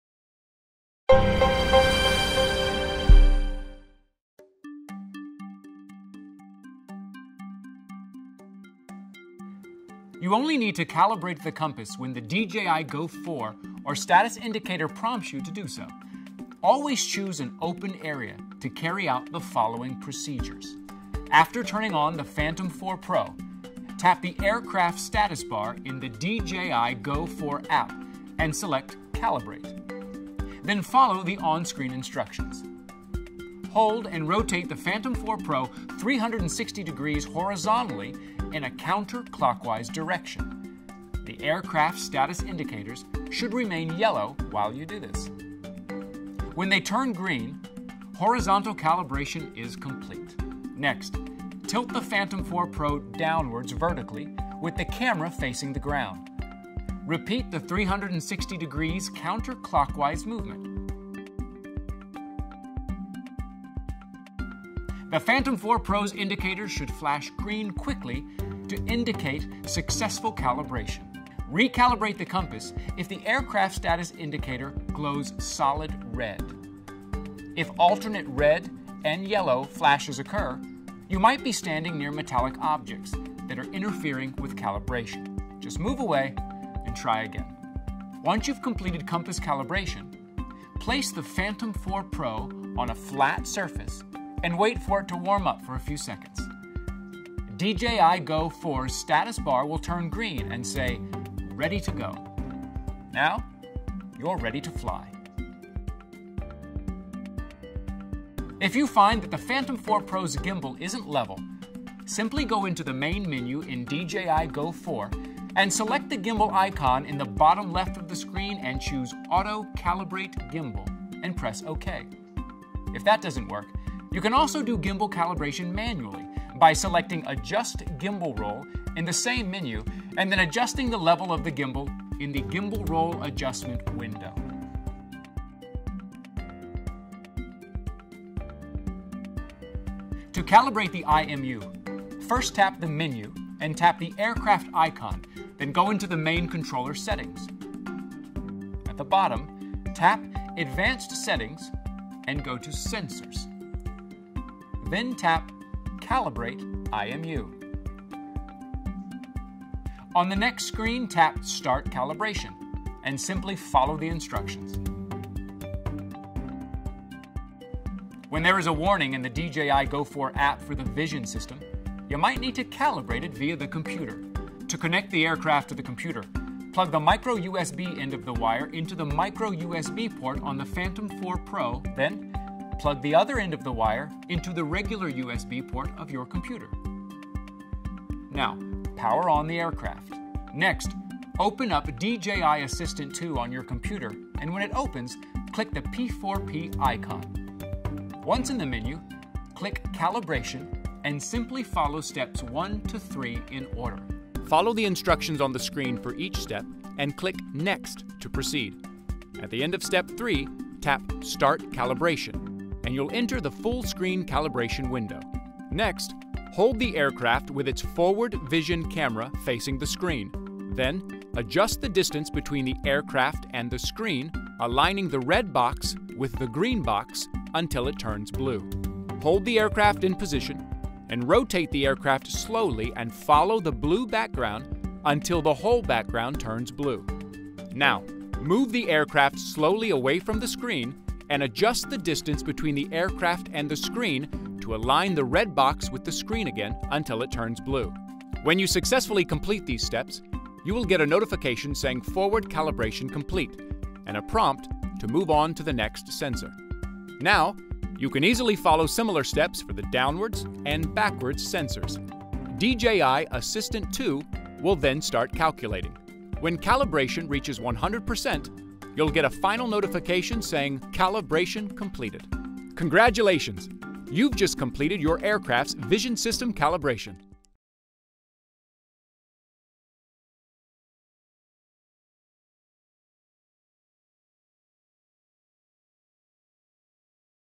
DJI Tutorials - Phantom 4 Pro - How to Fly - Calibration
You may be prompted to complete a Compass or Vision System calibration for your Phantom 4 Pro. If so, watch this video as DJI’s Ty walks you through the simple steps of these procedures.